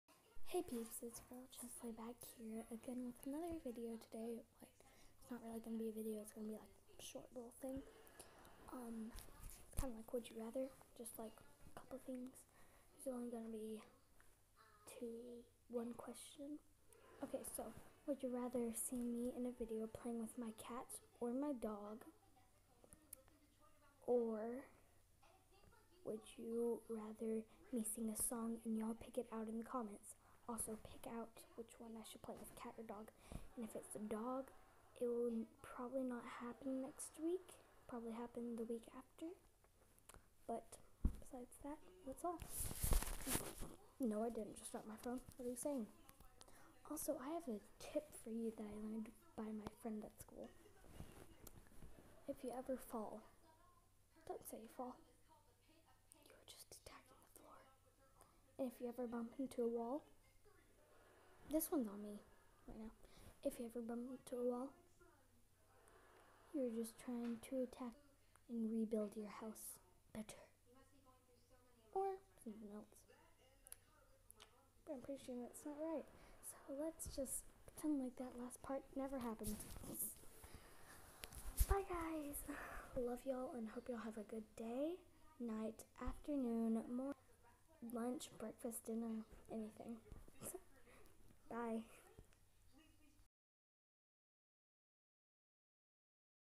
Plz help chose for meh plz
Would you wrather me sing a song and y'all pick it or me play with my cat or dog u choose